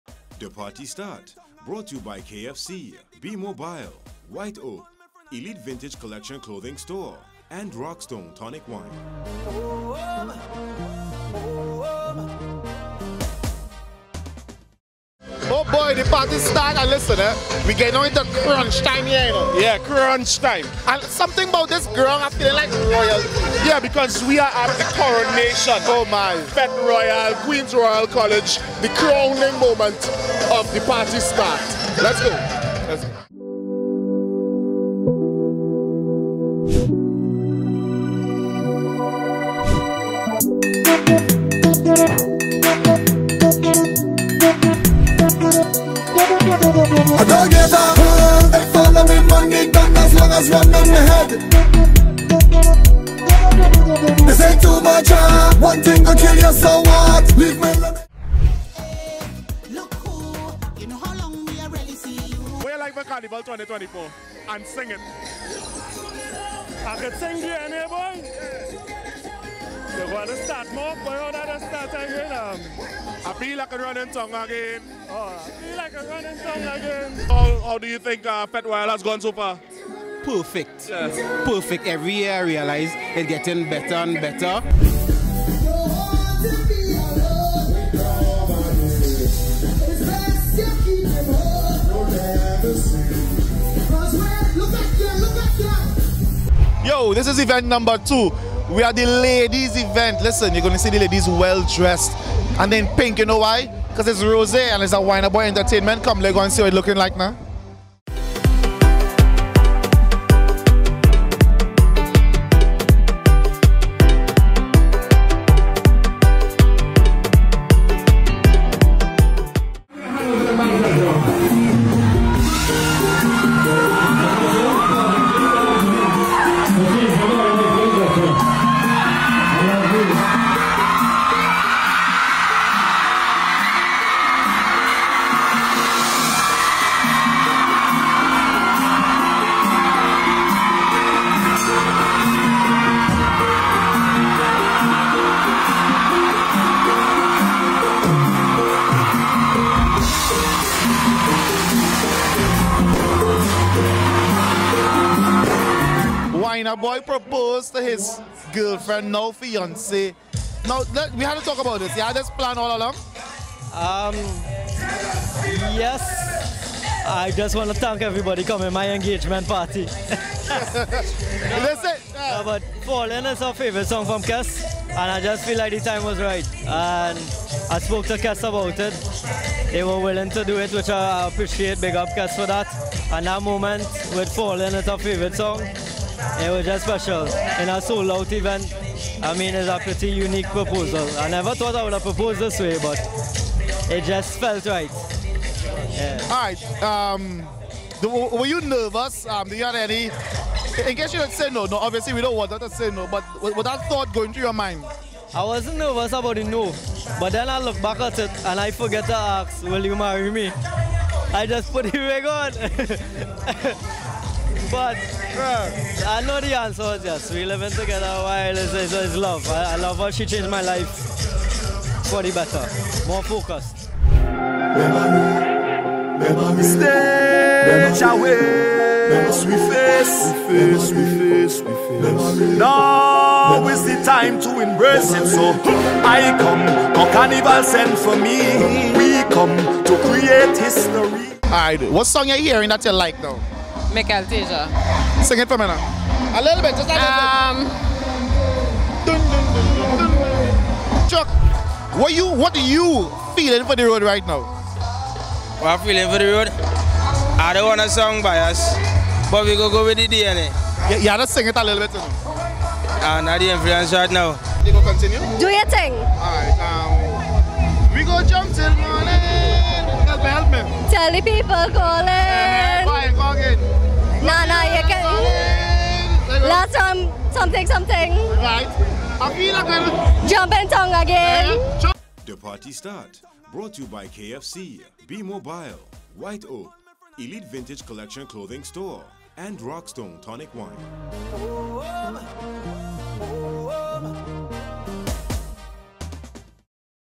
De Party Start
Carnival is a season of love ... this rang truest at Friday Night's Rose event where the fete's promoter created a memory for patrons and for himself.

De Party Start crew was there and also attended QRC's coronation where the Viking and Viqueen thrilled those in attendance.